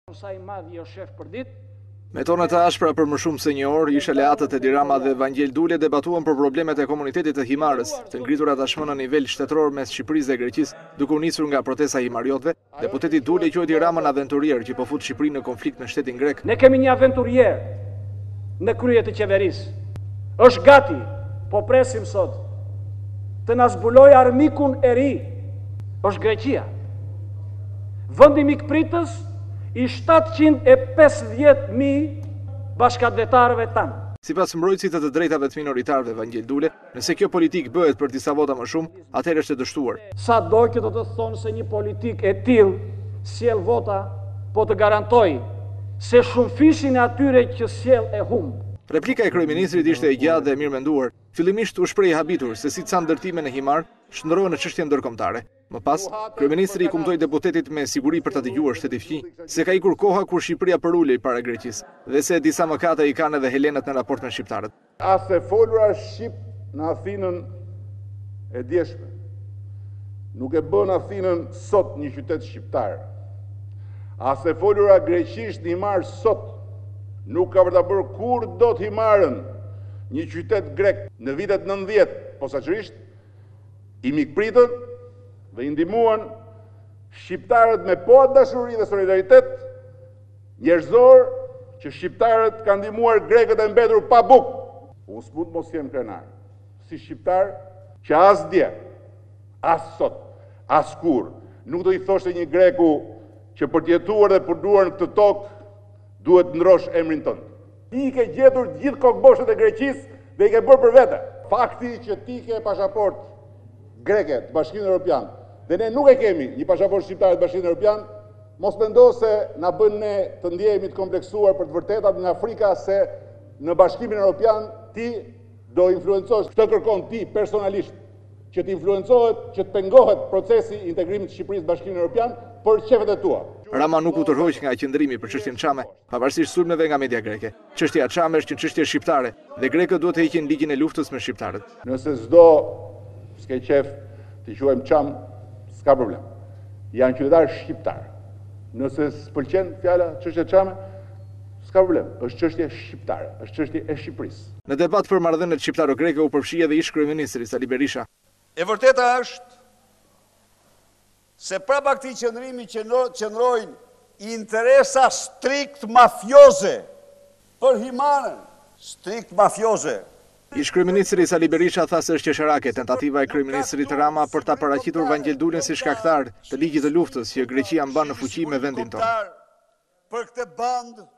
Ora News – Rama, Athinës e Dules: Himara shqiptare. Hiqni ligjin e Luftës
Me tone të ashpra për më shumë se 1 orë, ish aleatët Edi Rama dhe Vangjel Dule debatuan për problemet e komunitetit të Himarës të ngritura tashmë në nivel shtetëror mes Shqipërisë dhe Greqisë.
Duke u nisur nga protesta e himariotëve, deputeti Dule e quajti Ramën aventurier që po fut Shqipërinë në konflikt me shtetin grek.
Sipas mbrojtësit të të drejtave të minoritarëve, Vangjel Dule, nëse kjo politikë bëhet për disa vota më shumë, atëherë është e dështuar.
Replika  e Kryeministrit ishte e gjatë dhe e mirëmenduar. Fillimisht, u shpreh i habitur se si ca ndërtime në Himarë shndërrohen në çështje ndërkombëtare. Më pas, Kryeministri i kumtoi deputetit, me siguri për ta dëgjuar shteti fqinj, se ka ikur koha kur Shqipëria përulej para Greqisë, dhe se disa mëkate i kanë edhe helenët në raport me shqiptarët.
Rama i përmendi Dules pasaportën greke si një argument se po punon për  interes te partisë së tij dhe po mundohet të prishë marrëdhënien mes dy vendeve për të penguar integrimin e Shqipërisë.
Rama nuk u tërhoq nga qëndrimi për çështjen çame pavarësisht sulmeve nga media greke. Çështja çame është një çështje shqiptare dhe Grekët duhet të heqin ligjin e luftës me shqiptarët.